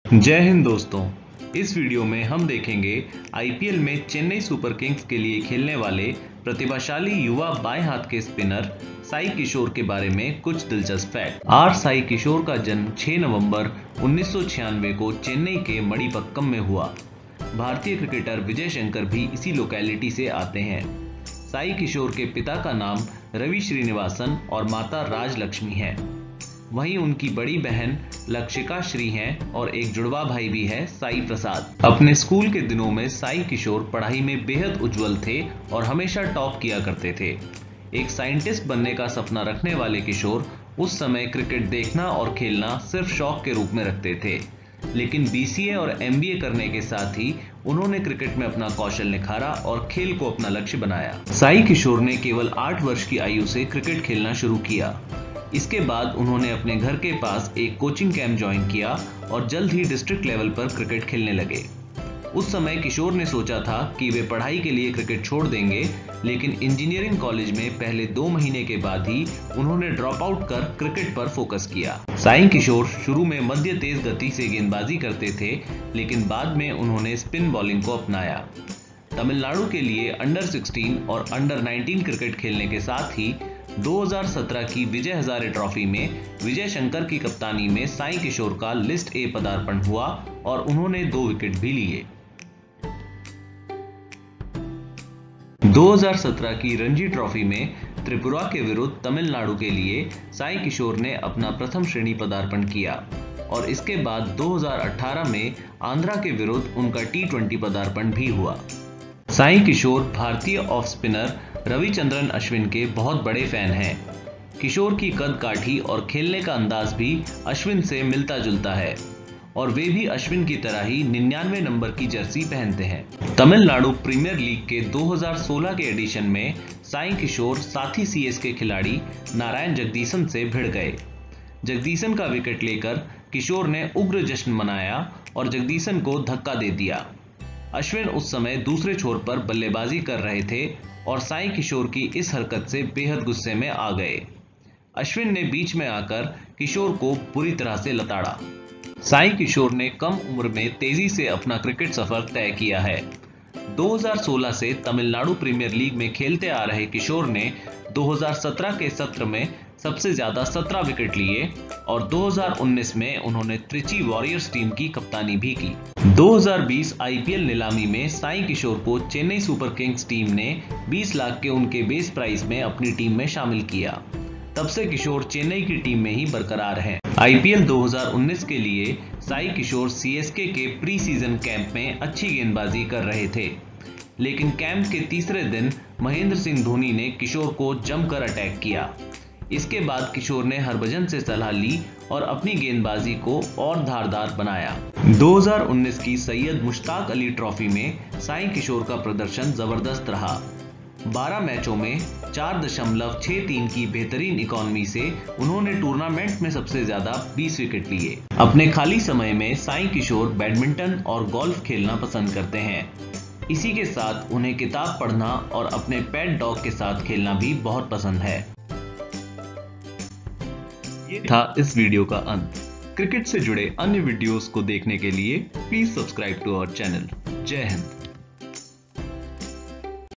🇮🇳 RSaiKishore Lifestyle😎, Wife, Salary, Net Worth, Biography & Top Facts Hindi CSK 🏏
आर साई किशोर का जन्म 6 नवम्बर, 1996 को चेन्नई के मडिपक्कम में हुआ। भारतीय क्रिकेटर विजय शंकर भी इसी लोकैलिटी से आते हैं। साई किशोर के पिता का नाम रविश्रीणिवासन और माता राजलक्ष्मी हैं। वहीँ उनकी बड़ी बहन लखशिखा श्री हैं और एक जुड़वाँ भाई भी हैं - साई प्रसाद।

अपने स्कूल के दिनों में साई किशोर पढाई में बेहद उज्जवल थे और हमेशा टॉप किया करते थे। एक साइंटिस्ट बनने का सपना रखने वाले किशोर उस समय क्रिकेट देखना और खेलना सिर्फ शौक के रूप में रखते थे। लेकिन BCA और MBA करने के साथ ही उन्होंने क्रिकेट में अपना कौशल निखारा और खेल को अपना लक्ष्य बनाया।

साई किशोर ने केवल 8 वर्ष की आयु से क्रिकेट खेलना शुरू किया। इसके बाद उन्होंने अपने घर के पास एक कोचिंग कैंप ज्वाइन किया और जल्द ही डिस्ट्रिक्ट लेवल पर क्रिकेट खेलने लगे। उस समय किशोर ने सोचा था की वे पढाई के लिए क्रिकेट छोड़ देंगे लेकिन इंजीनियरिंग कॉलेज में पहले 2 महीने के बाद ही उन्होंने ड्रॉपआउट कर क्रिकेट पर फोकस किया।

साई किशोर शुरू में मध्य तेज़ गति से गेंदबाज़ी करते थे लेकिन बाद में उन्होंने स्पिन बोलिंग को अपनाया। तमिल नाडु के लिए अंडर-16 और अंडर-19 क्रिकेट खेलने के साथ ही 2017 की विजय हज़ारे ट्रॉफी में विजय शंकर की कप्तानी में साई किशोर का लिस्ट A पदार्पण हुआ और उन्होंने 2 विकेट भी लिए।

2017 की रणजी ट्रॉफी में त्रिपुरा के विरुद्ध तमिल नाडु के लिए साई किशोर ने अपना प्रथम श्रेणी पदार्पण किया और इसके बाद 2018 में आंध्र के विरुद्ध उनका टी२० पदार्पण भी हुआ।

साई किशोर भारतीय ऑफ स्पिनर रविचंद्रन अश्विन के बहुत बड़े फैन हैं। किशोर की कद काठी और खेलने का अंदाज़ भी अश्विन से मिलता जुलता है और वे भी अश्विन की तरह ही 99 नंबर की जर्सी पहनते हैं।

तमिल नाडु के 2016 एडिशन में साई किशोर साथी सीएसके खिलाड़ी नारायण जगदीसन से भिड़ गए। जगदीसन का विकेट लेकर किशोर ने उग्र जश्न मनाया और जगदीसन को धक्का दे दिया। अश्विन उस समय दूसरे छोर पर बल्लेबाज़ी कर रहे थे और साई किशोर की इस हरकत से बेहद गुस्से में आ गए। अश्विन ने बीच में आकर किशोर को बुरी तरह से लताड़ा।

साई किशोर ने कम उम्र में तेज़ी से अपना क्रिकेट सफर तय किया है। 2016 से तमिल नाडु प्रीमियर लीग में खेलते आ रहे किशोर ने 2017 के सत्र में सबसे ज़्यादा 17 विकेट लिए और 2019 में उन्होंने त्रिची वारियर्स टीम की कप्तानी भी की।

2020 आईपीएल नीलामी में साई किशोर को चेन्नई सुपरकिंग्स टीम ने 20 लाख के उनके बेस प्राइस में अपनी टीम में शामिल किया। तब से किशोर चेन्नई की टीम में ही बरक़रार हैं।

आईपीएल 2019 के लिए साई किशोर सीएसके के प्री सीजन कैंप में अच्छी गेंदबाज़ी कर रहे थे। लेकिन कैंप के तीसरे दिन महेंद्र सिंह धोनी ने किशोर को जम कर अटैक किया। इसके बाद किशोर ने हरभजन से सलाह ली और अपनी गेंदबाज़ी को और धारधार बनाया।

2019 की सैयद मुश्ताक़ अली ट्रॉफी में साई किशोर का प्रदर्शन ज़बरदस्त रहा। 12 मैचों में 4.63 की बेहतरीन इकॉनमी से उन्होंने टूर्नामेंट में सबसे ज़्यादा 20 विकेट लिए।

अपने खाली समय में साई किशोर बैडमिंटन और गोल्फ खेलना पसंद करते हैं। इसी के साथ उन्हें किताब पढ़ना और अपने पेट डॉग के साथ खेलना भी बहुत पसंद है।